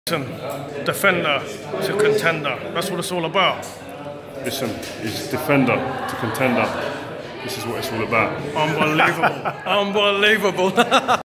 When Rio Ferdinand met "Rio Ferdinand"
Doing my Rio impression in front of the man himself!